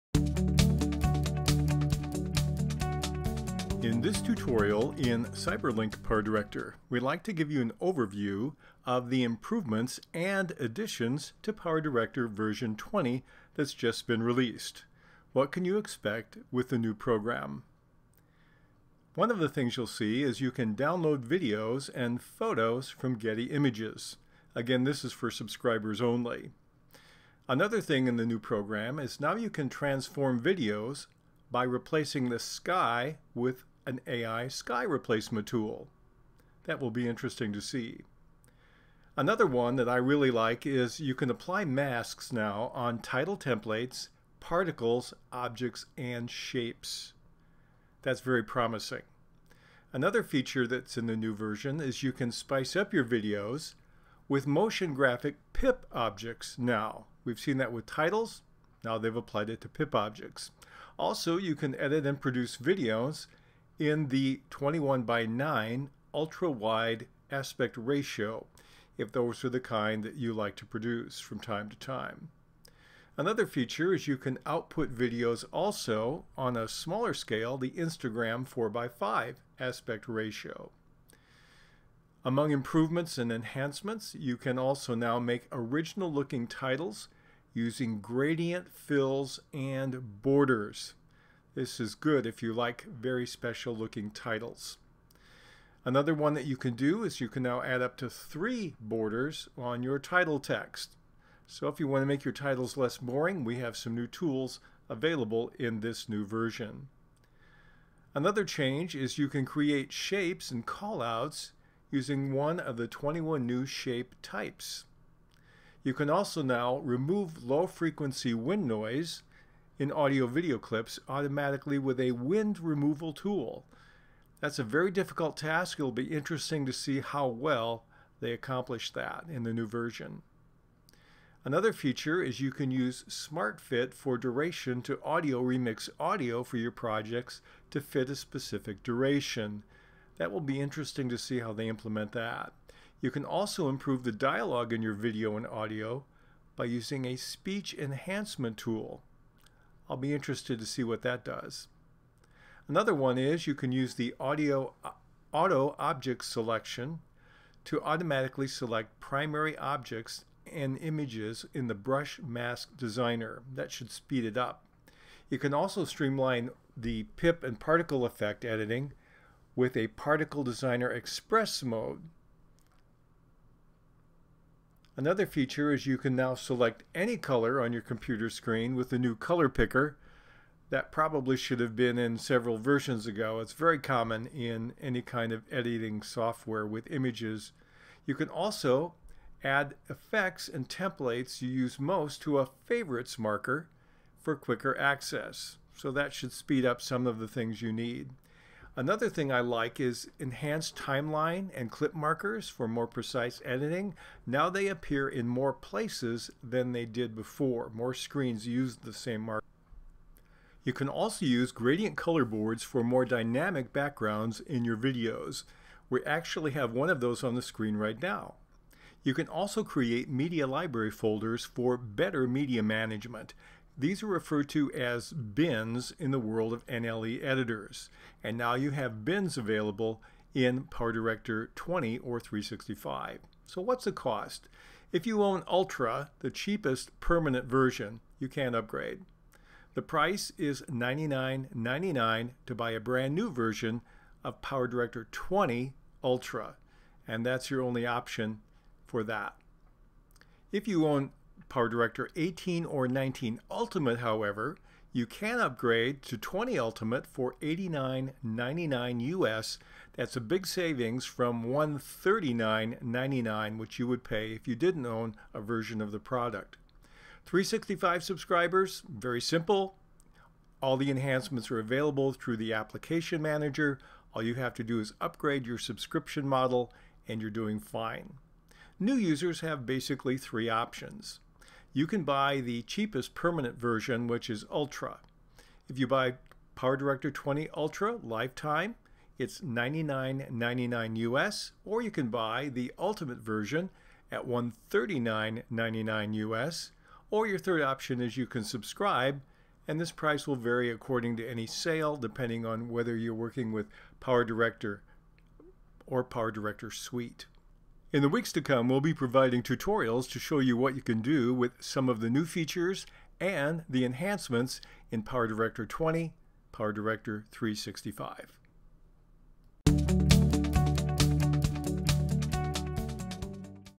PowerDirector - Preview of the new PowerDirector 20 / 365
We take a quick look at the improvements and enhancements to the newly-released version of PowerDirector. We will cover these features in detail in upcoming tutorials.